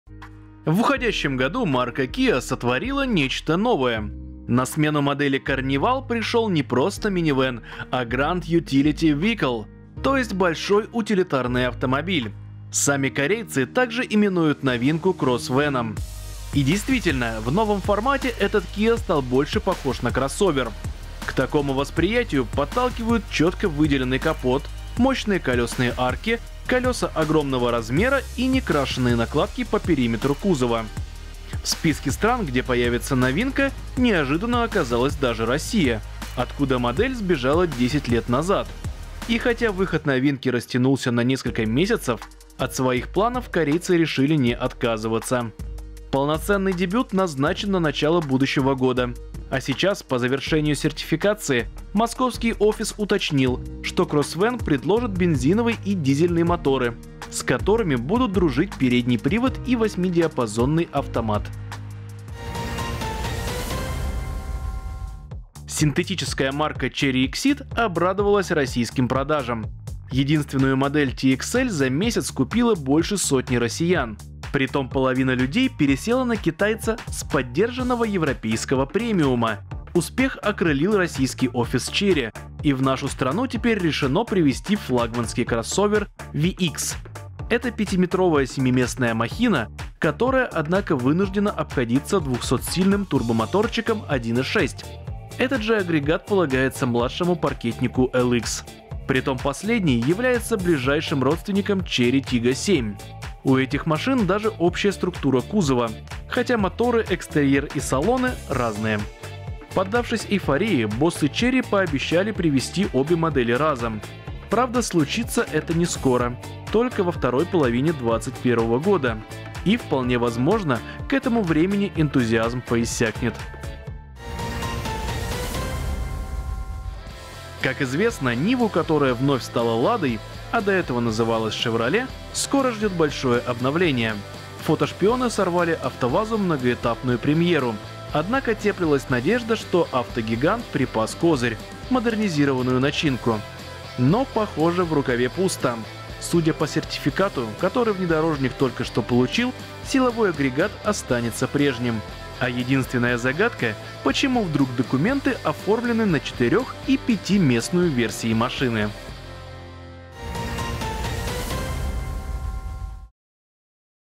Kia Carnival и Lada Niva сертифицированы, CheryExeed VX и LX приедут в 2021 | Новости с колёс №1294
Четвёртое поколение Киа Карнивал названо не просто минивэном, а Grand Utility Vehicle — «большим утилитарным автомобилем». В Россию этот кроссовера и вэна приедет в начале 2021 года. Сертификация завершена: «кроссвэн» предложит бензиновый и дизельный моторы.

Синтетическая марка Cheryexeed обрадовалась российским продажам. Единственную модель TXL за месяц купило больше сотни россиян. Теперь решено привезти к нам флагманский кроссовер VX, а заодно и младший паркетнику Exeed LX. Правда, случится это нескоро, только во второй половине 2021 года.

Как известно, Ниву, которая вновь стала Ладой, а до этого называлась Chevrolet, скоро ждёт большое обновление. Судя по сертификату, который внедорожник только что получил, силовой агрегат останется прежним. А единственная загадка — почему вдруг документы оформлены на четырёх- и пятиместную версии машины?